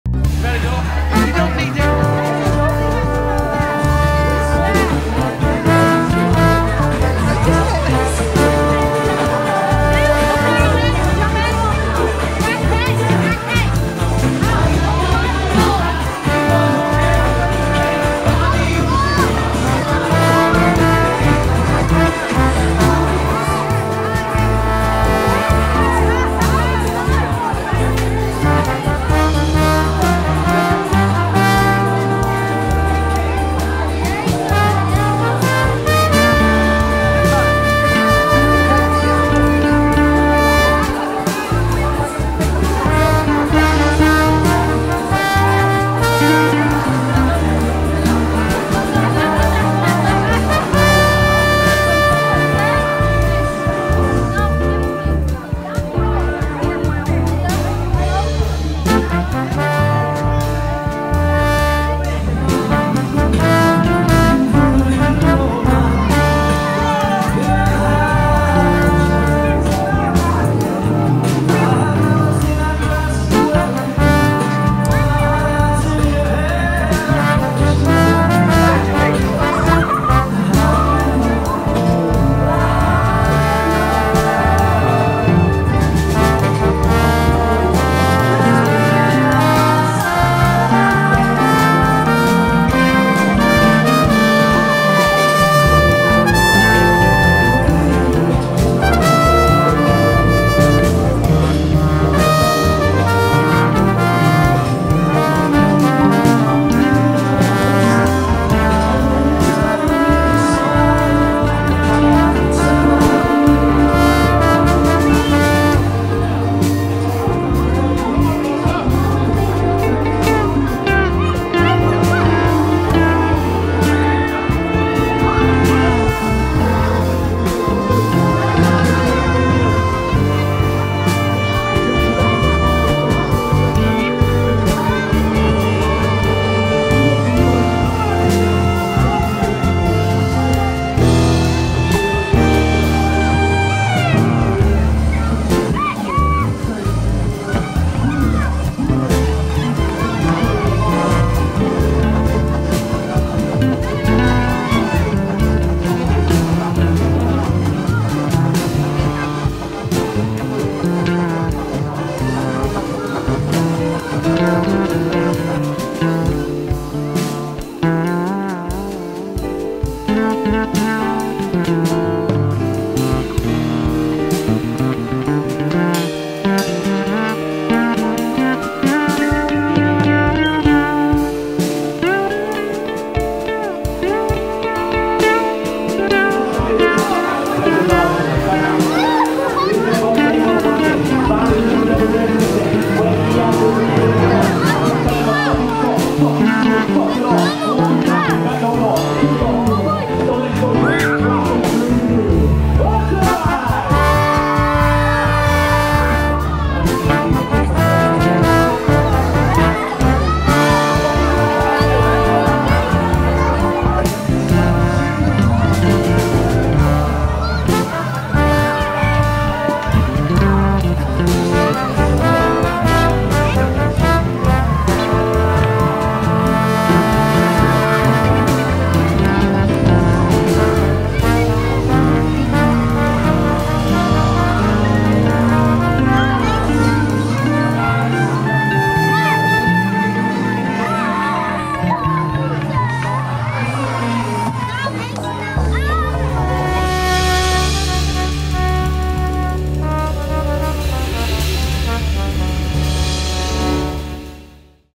Gypsy Wedding Cake Falls Over
This all happened so quickly. The bride and groom stood by the wedding cake, both their hands encircling the knife.  Next minute there was mayhem - a total free-for all!   In te end I had to leave as the kids were getting totally out of hand...